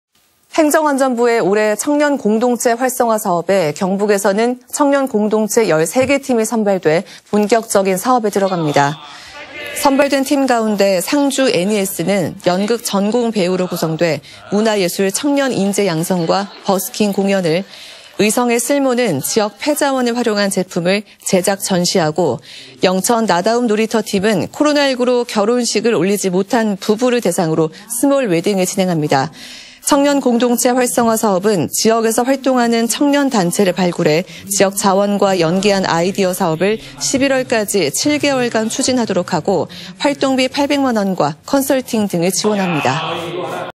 경북 올해 청년공동체 활성화사업 13개팀 운영 / 안동MBC
행정안전부의 올해 청년공동체 활성화사업에
경북에서는 청년공동체 13개팀이 선발돼
본격적인 사업에 들어갑니다.
           ◀ＥＮＤ▶

선발된 팀 가운데 상주 '엔이에스'는
연극 전공 배우로 구성돼
문화예술 청년 인재 양성과 버스킹 공연을,
의성의 '쓸모'는 지역 폐자원을 활용한 제품을
제작·전시, 영천 '나다움놀이터'팀은
코로나19로 결혼식을 올리지 못한 부부를
대상으로 스몰웨딩을 진행합니다.

청년공동체 활성화사업은
지역에서 활동하는 청년단체를 발굴해
지역 자원과 연계한 아이디어 사업을
11월까지 7개월간 추진하도록 하고,
활동비 800만 원과 컨설팅 등을 지원합니다.